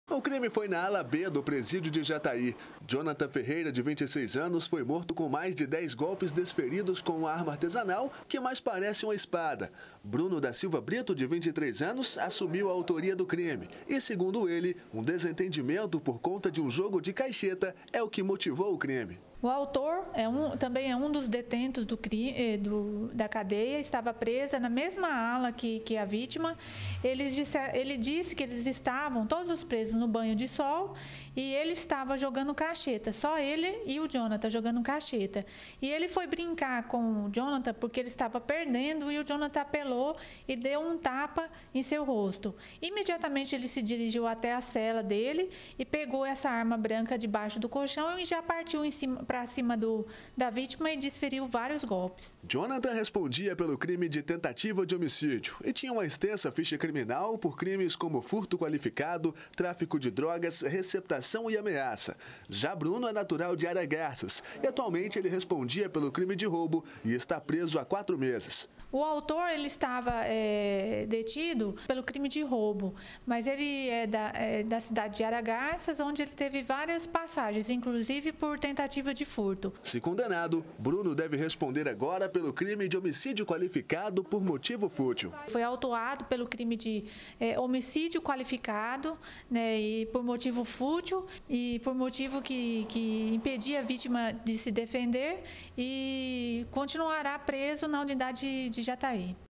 BG - Morte dentro do presídio de Jataí - 24-10-2017
Um preso de 26 anos foi morto dentro do presídio de Jataí, no sudoeste goiano. Um colega de 23 anos teria matado o jovem usando uma arma artesanal. Um jogo de caixeta teria motivado o crime.